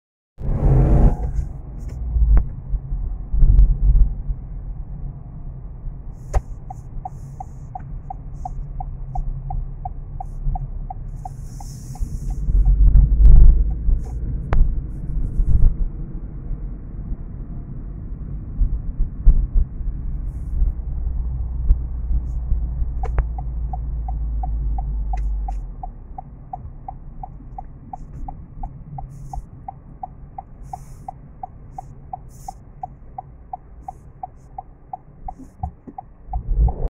A short ride with a view of the Saski Palace and other buildings
You can see the building of Saski Palace and old tenement houses. You can also see trees and traffic. You can hear the sound of traffic, the sound of the engine and the sound of the turn signal. This video was taken in Kutno (Lodzkie Voivodeship, Poland).